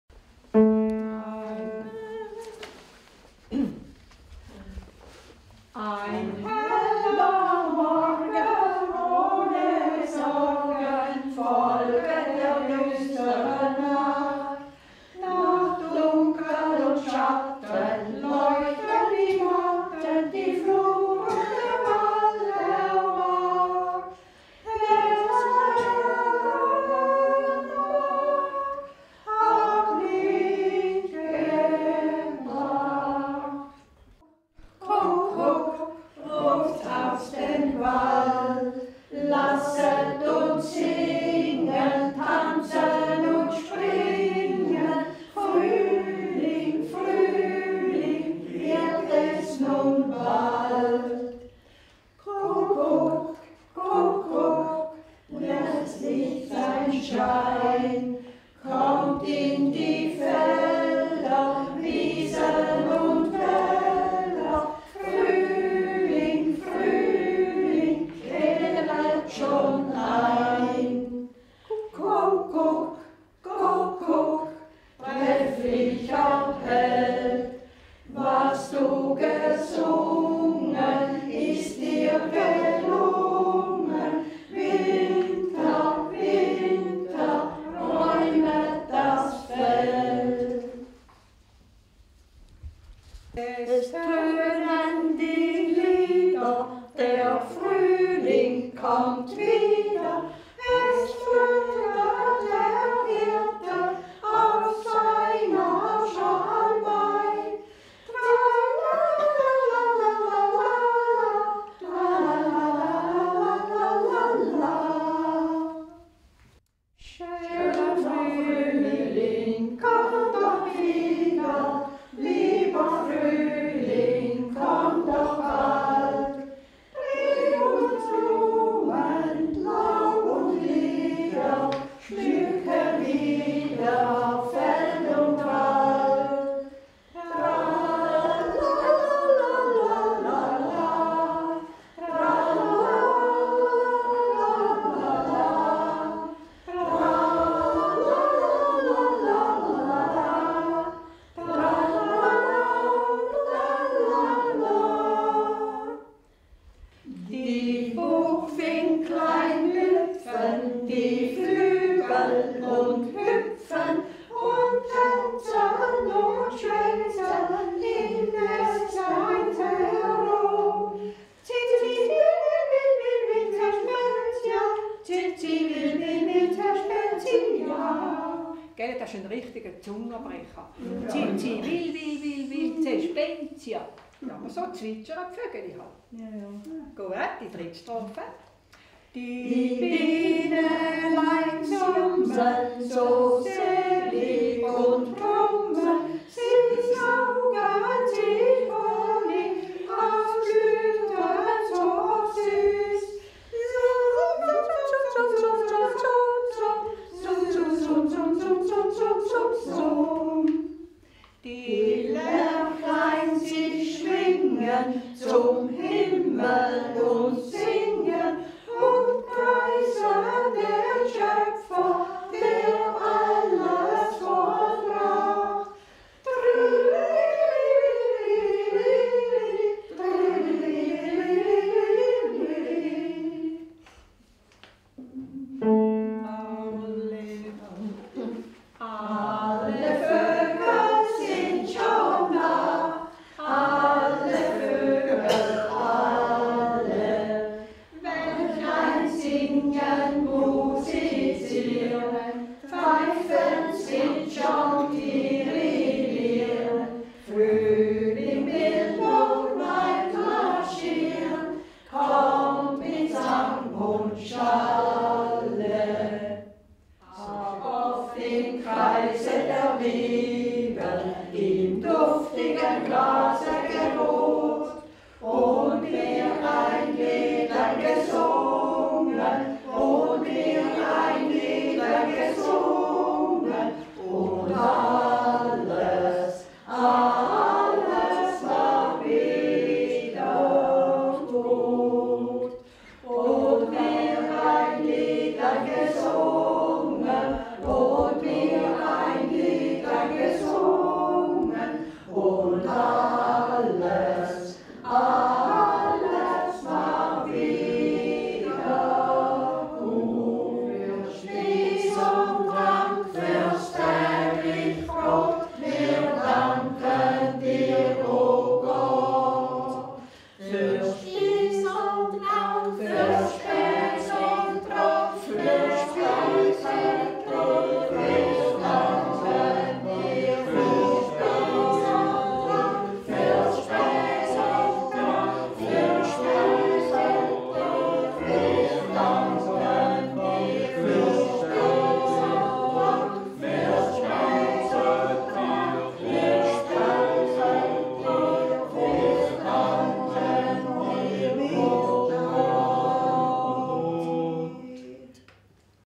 Singen im Lindenhügel
Im Alters- und Pflegeheim Haus Lindenhügel wird einmal im Monat miteinander gesungen. Unter Anleitung von Elisabeth Bosshard werden nicht nur alte, kaum mehr gesungene Lieder zu neuem Leben erweckt, auch manche Erinnerungen tauchen wieder auf an die Schulzeit.